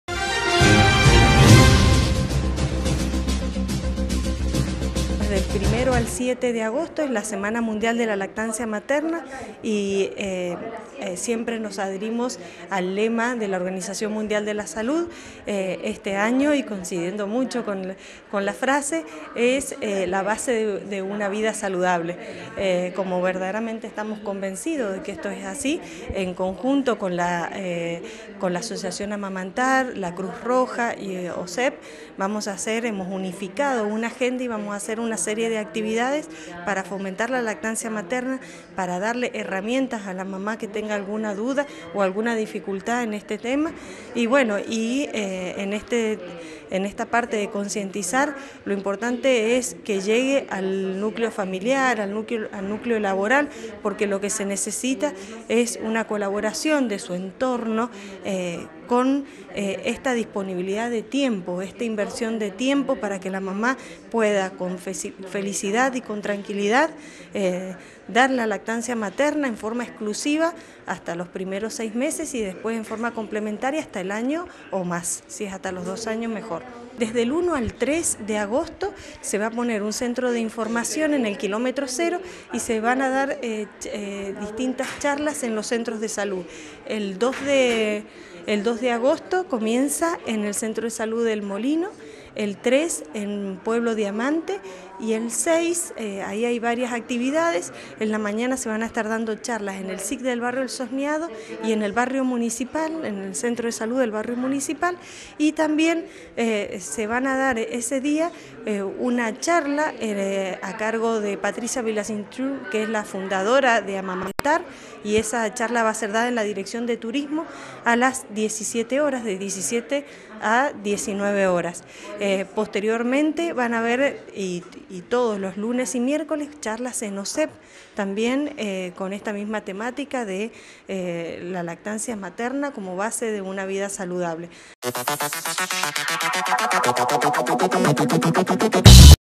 LA BASE DE UNA VIDA SALUDABLE
La Directora de Salud Municipal Paola Calle explicó que la lactancia materna es la base de una vida saludable, por ello, en el marco de la Semana Mundial de la Lactancia Materna se realizarán charlas en centros de salud, comenzando el día 2 de agosto en el Centro de salud del Barrio "El Molino", 3 de agosto en "Pueblo Diamante" y el día 6 en la mañana en el SIC del Barrio El Sosneado y en Barrio Municipal y por la tarde, la charla de la Presidente de la Asociación Amamantarg a las 19hs en la oficina de turismo de nuestro departamento.